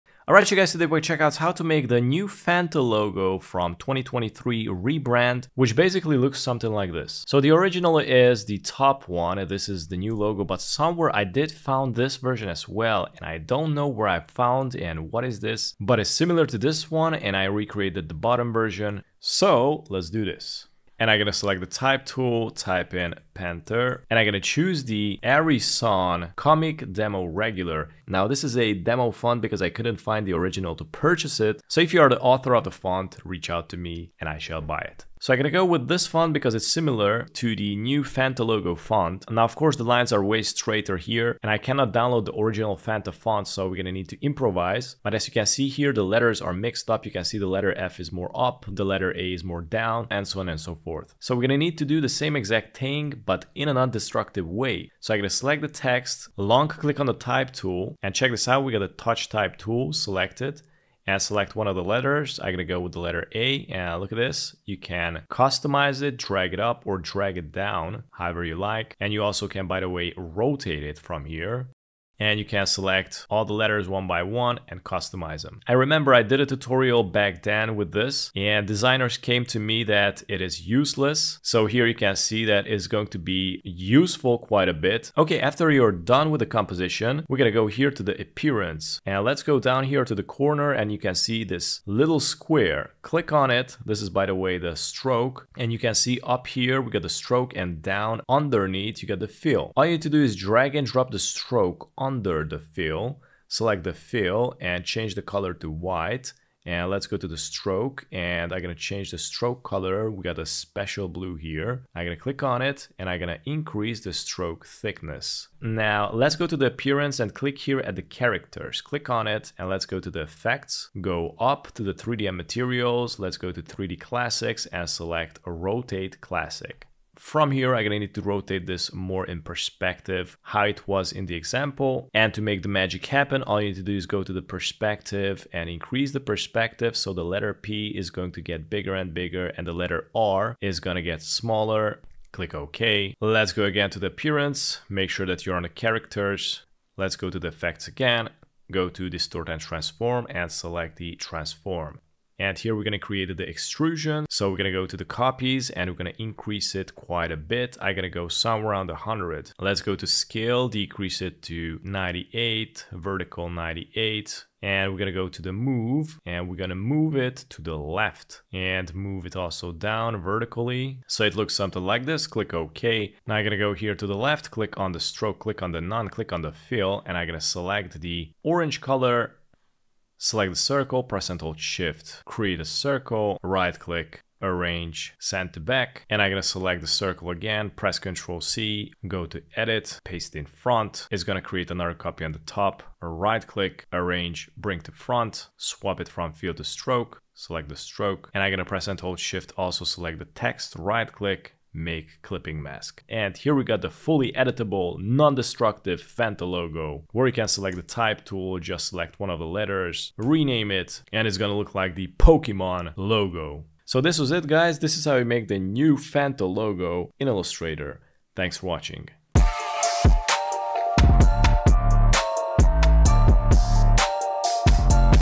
How To Design The NEW Fanta Logo In Illustrator
Fanta redesigned their logo last year in 2023 and the newest logo is more in perspective with a long extrusion and the letters are straight sans serif types where the letters are inconsistent and arranged very nicely in an informal matter. In this tutorial you gonna learn how to make a 3D extruded non-destructive Fanta logo in Illustrator which you can rename any time.